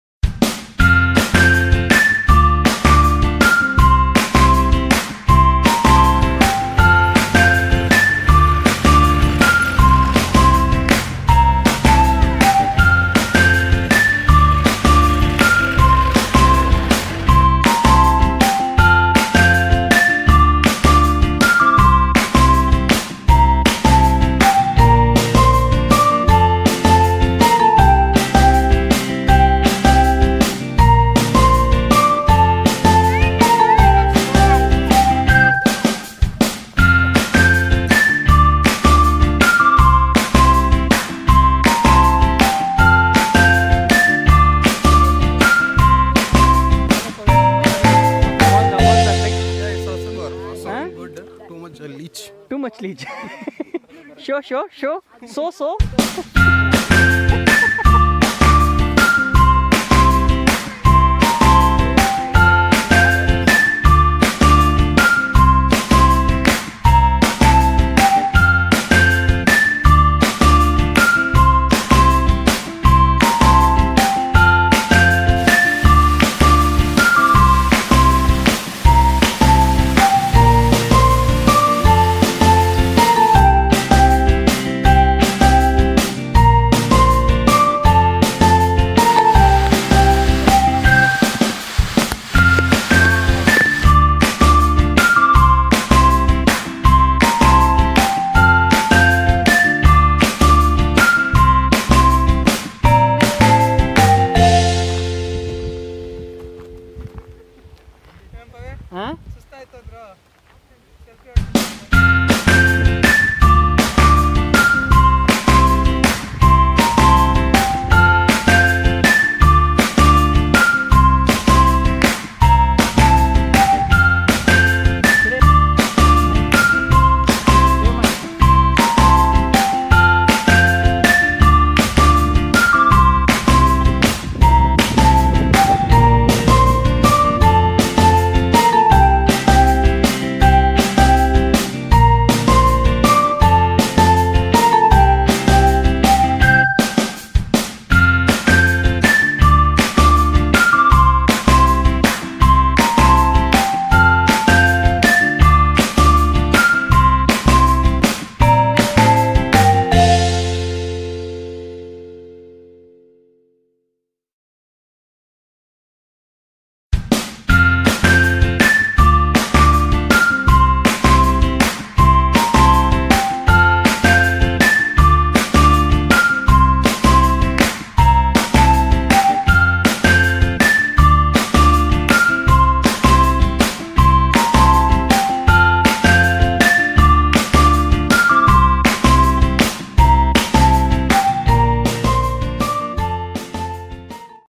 Event#25: Trek to Kodachadri
“It feels good to be lost in the right direction with a troupe of like-minded!”

The Kodachadri planned on 2018 Oct 27th & 28th was very well executed without any delay, anytime, anywhere by a bunch of adventure seekers who were very punctual and enthusiastic. All were on time to board the vehicle, start the trek, reached the peak within a decent time crossing streams, paddy fields, passing through the forest terrain, getting few leech bite, dipping under the waterfalls, climbing the steep rocky terrain & grasslands.

Kudos to our young trekkers Danyath and Shresht who were the first to reach the peak, setting milestone to others.

We must say again it was a wonderful team we met with.